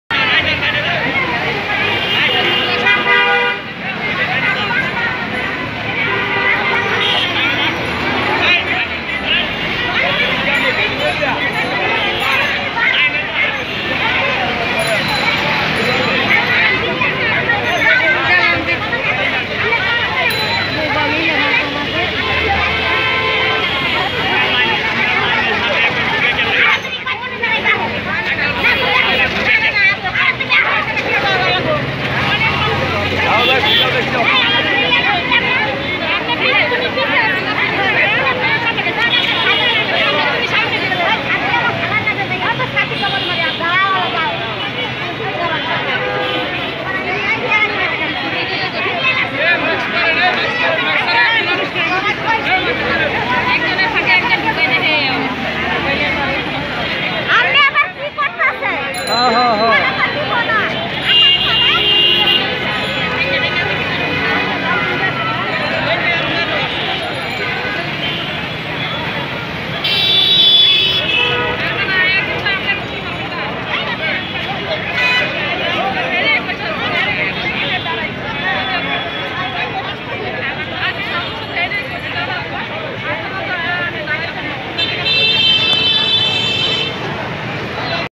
টিসিবির পণ্য কিনতে নিন্মবিত্তদের দীর্ঘ সারি
মঙ্গলবার (১৩ জুলাই ) সকালে রাজধানীর মহাখালীতে টিসিবির পণ্য কিনতে করোনায় বিপর্যস্তদের দীর্ঘ সারি।